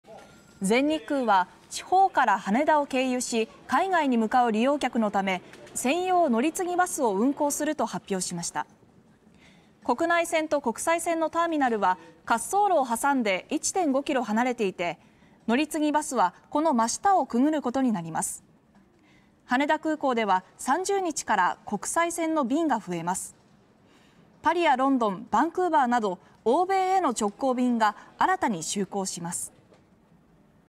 羽田空港 欧米直行便新就航で国内線乗り継ぎ便利に(14/03/17)
羽田空港の国際線が増便することを受けて、国内線との乗り継ぎが便利になります。　全日空は、地方から羽田を経由し、海外に向かう利用客のため、専用乗り継ぎバスを運行すると発表しました。